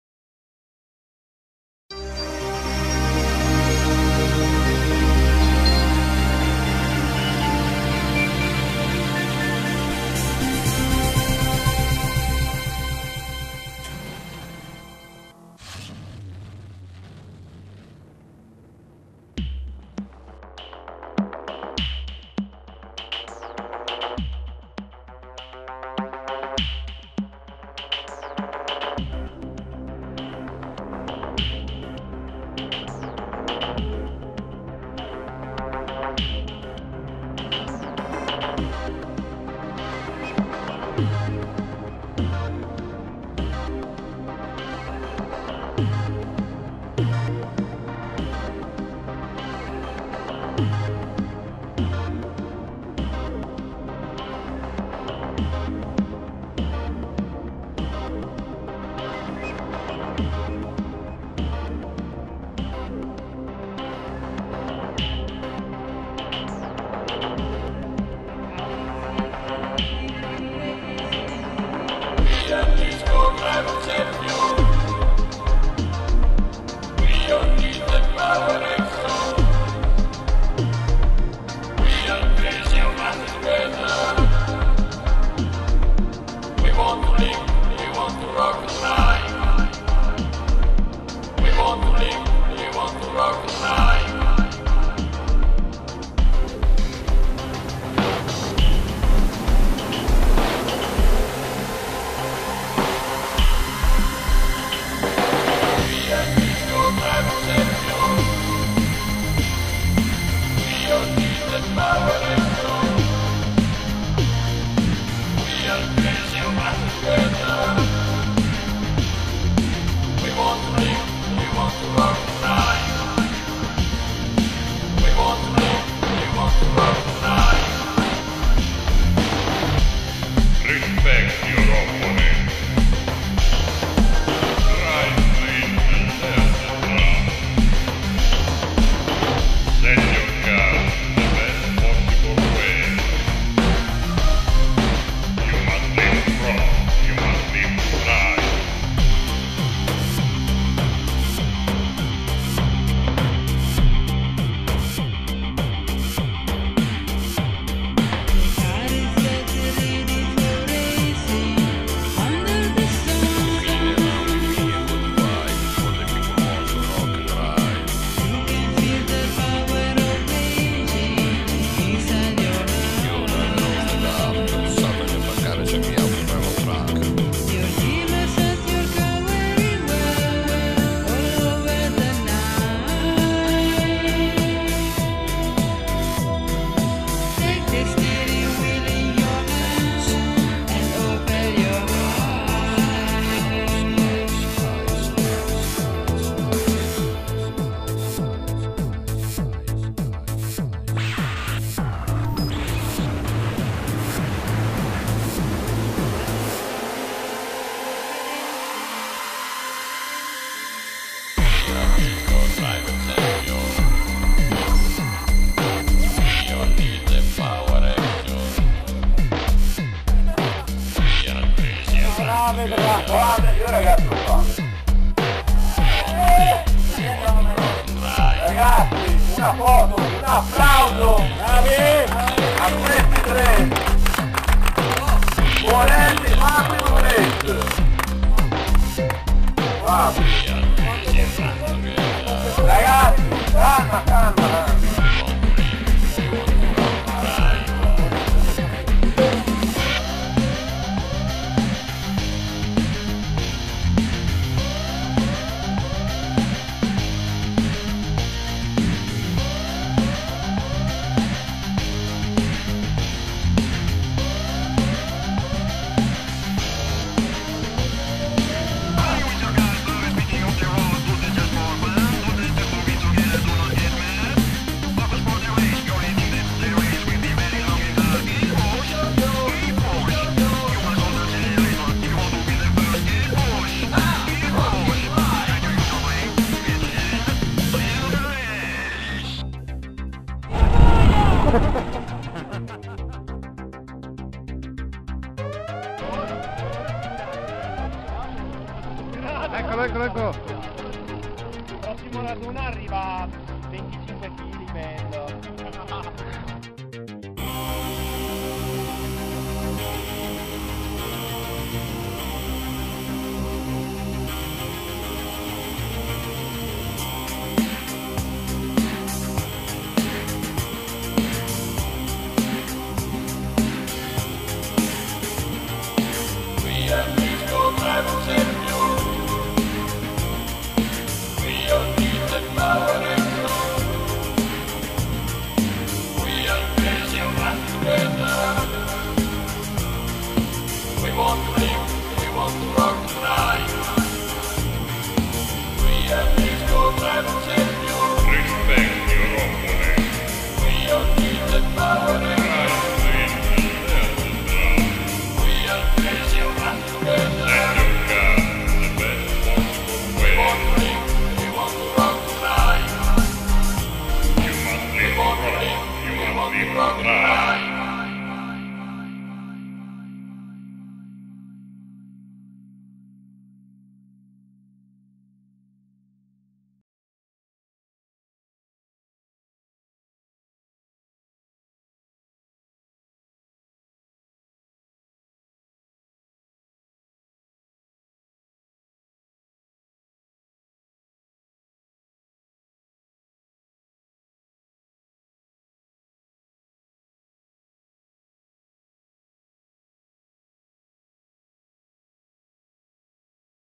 Lisgo Rock And Drive
E' un riassunto delle puntate precedenti, con immagini e video prese qua e là in Lisgo.
Perdonate chi canta :)))